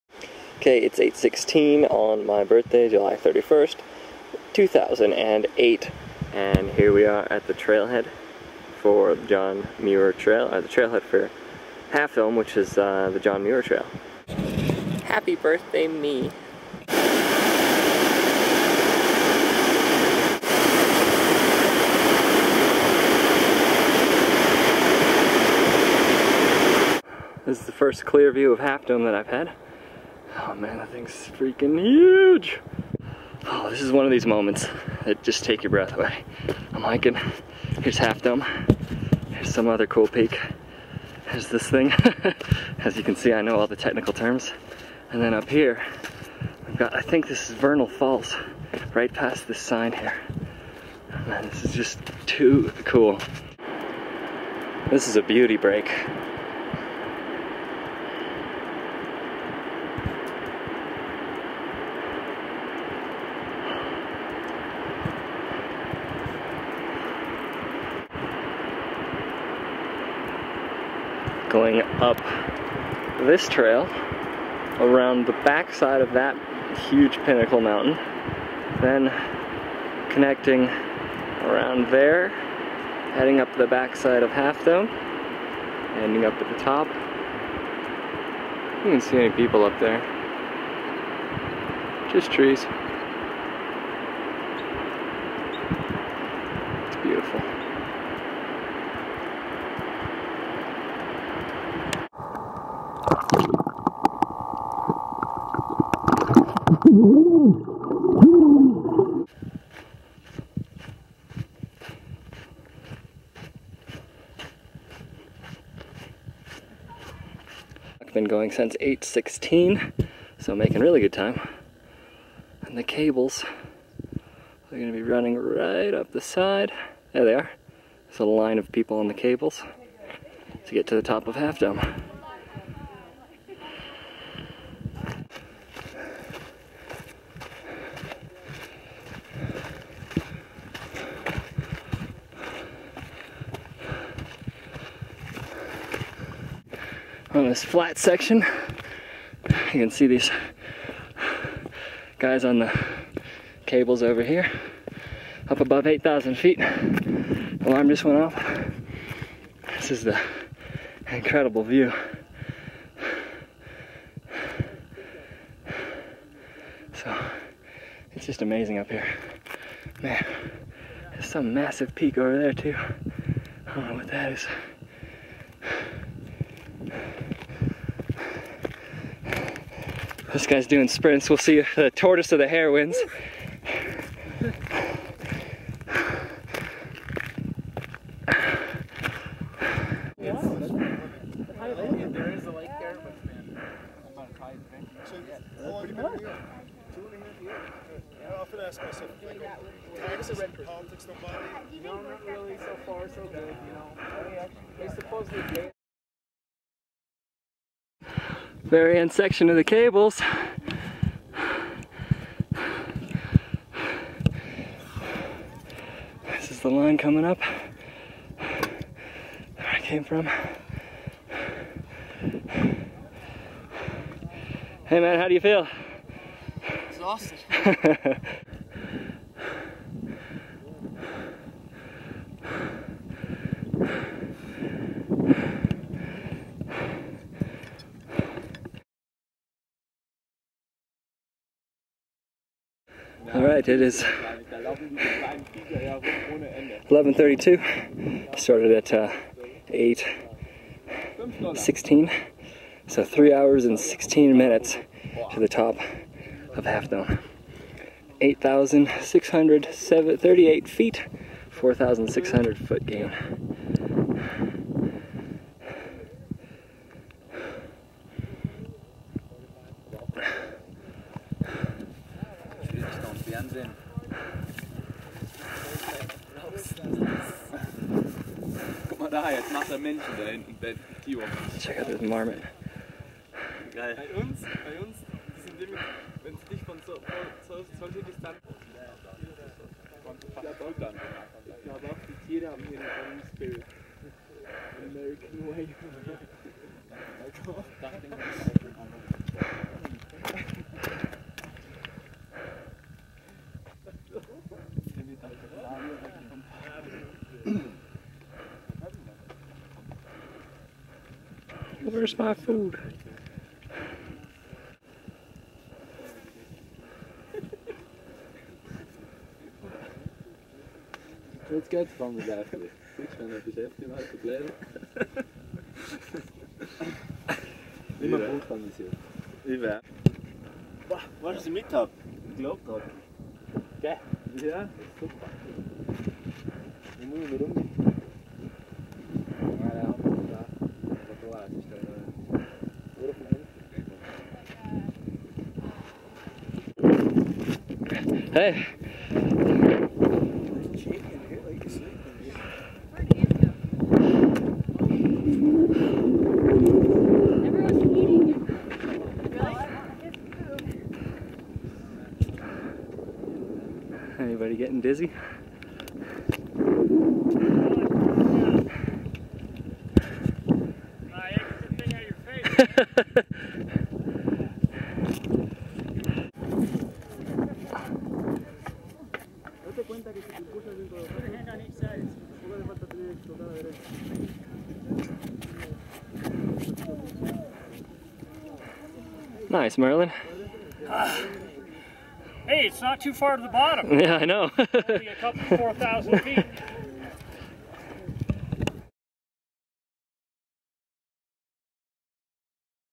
Half Dome Hike in Yosemite
- hiking from Yosemite's valley floor to the summit of half dome along the John Muir Trail. Hiking half dome took me 3 hours and 16 minutes.

ARE PERMITS REQUIRED?